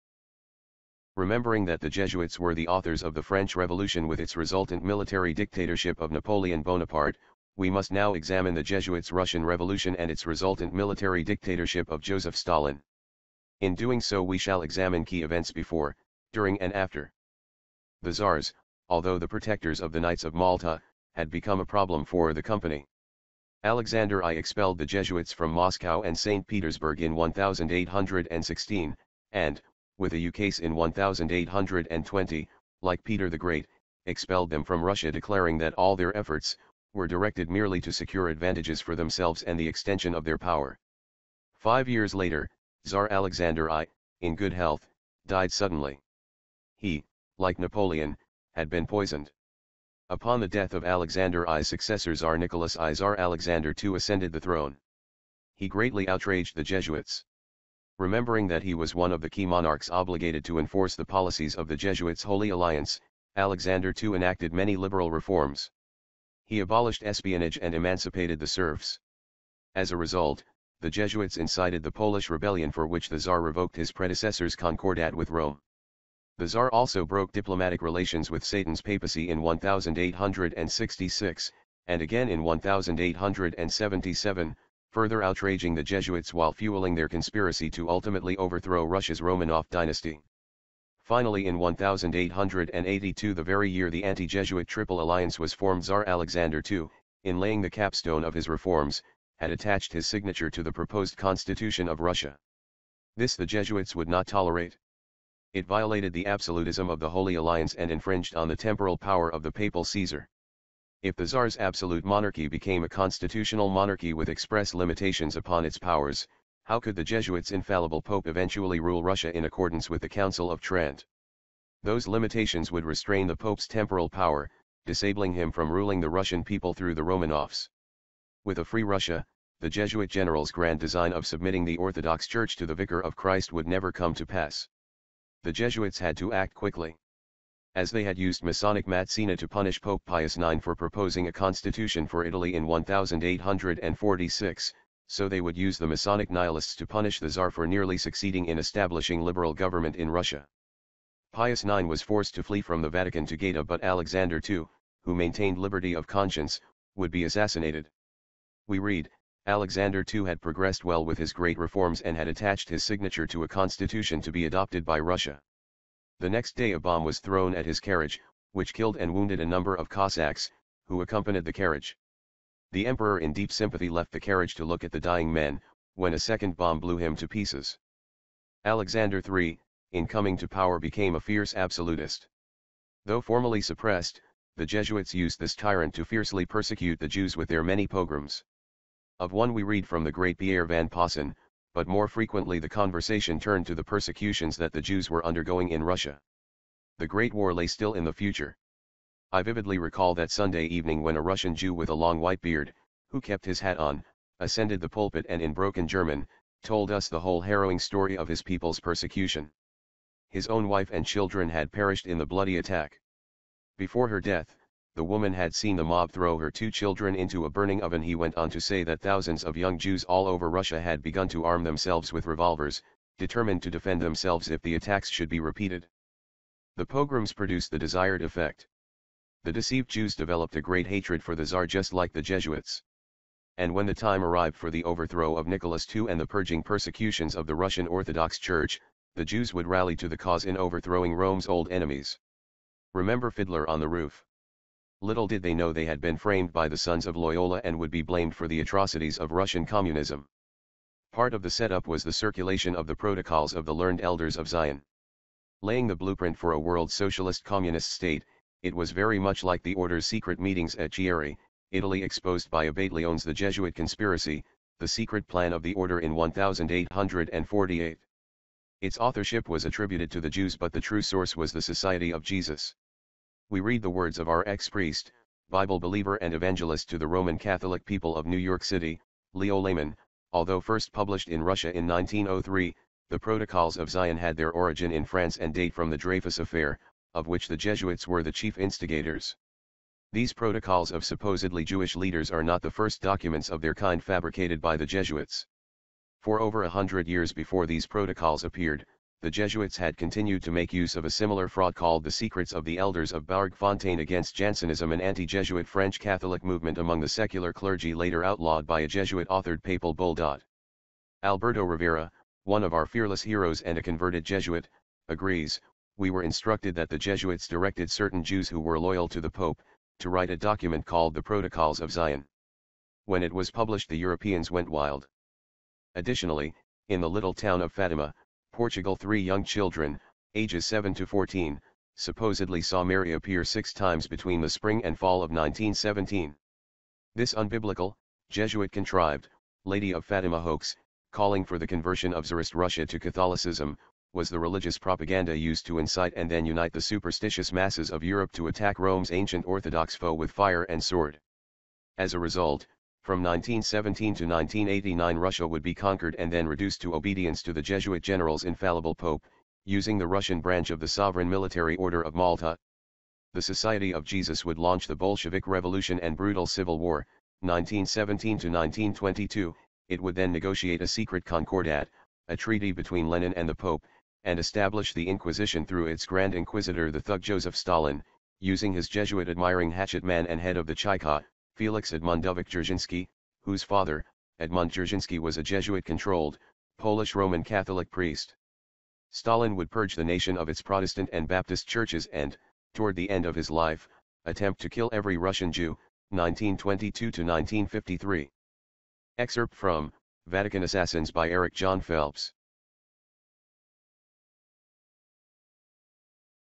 The Jewish Patsies : real origin of : The Protocols of the Learned Elders of Zion
Excerpt from : Vatican Assassins By Eric Jon Phelps
Remembering that the Jesuits were the authors of the French Revolution with its resultant military dictatorship of Napoleon Bonaparte, we must now examine the Jesuits’ Russian Revolution and its resultant military dictatorship of Joseph Stalin.
Tzar — just like the Jesuits! And when the time arrived for the overthrow of Nicholas II and the purging persecutions of the Russian Orthodox Church, the Jews would rally to the cause in overthrowing Rome’s old enemies. (Remember Fiddler on the Roof ?) Little did they know they had been “framed” by the Sons of Loyola and would be blamed for the atrocities of Russian communism. Part of the set-up was the circulation of The Protocols of the Learned Elders of Zion. Laying the blueprint for a world socialist-communist state, it was very much like the Order’s secret meetings at Chieri, Italy exposed by Abate Leone’s The Jesuit Conspiracy: The Secret Plan of the Order in 1848. Its authorship was attributed to the Jews but the true source was the Society of Jesus. We read the words of our ex-priest, Bible believer and evangelist to the Roman Catholic people of New York City, Leo Lehmann: “Although first published in Russia in 1903, the Protocols of Zion had their origin in France and date from the Dreyfus Affair, of which the Jesuits were the chief instigators . . . These Protocols of supposedly Jewish leaders are not the first documents of their kind fabricated by the Jesuits. For over a hundred years before these Protocols appeared, the Jesuits had continued to make use of a similar fraud called The Secrets of the Elders of Bourg-Fontaine against Jansenism — an anti-Jesuit French Catholic movement among the secular clergy [later outlawed by a Jesuit-authored papal Bull].” Alberto Rivera, one of our fearless heroes and a converted Jesuit, agrees: “We were instructed that the Jesuits directed certain Jews who were loyal to the Pope, to write a document called The Protocols of Zion. When it was published the Europeans went wild.”
The True Authorship Of The New Testament  :
Arius Calpurnius Piso PEN NAME : FLAVIUS JOSEPHUS
The Nerva–Antonine dynasty was a dynasty of seven Roman Emperors who ruled over the Roman Empire from 96 AD to 192 AD